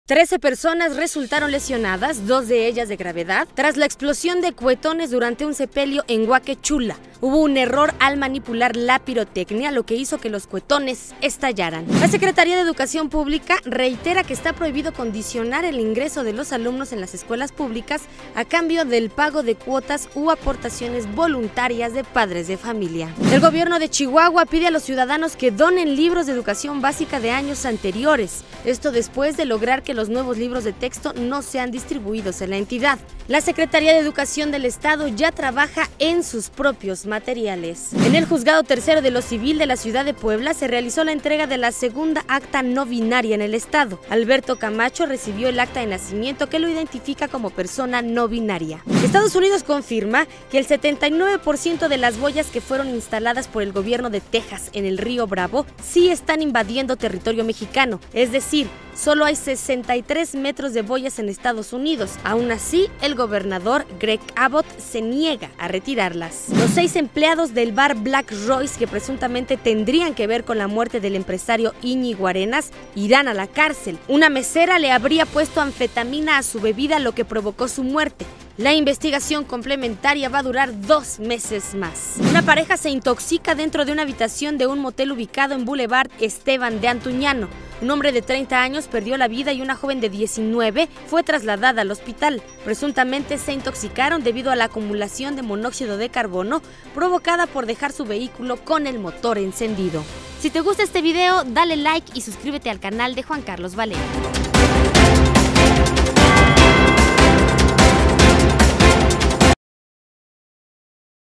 Resumen informativo 17 de agosto 2023 || Noticias con Juan Carlos Valerio
13 heridos, dos de ellos de gravedad tras explosión de cohetones en Huaquechula 🚨// Entregan segunda acta de nacimiento a una persona con género no binario en Puebla // Trabajadores de bar Black Royce se quedan en la cárcel. ⚠️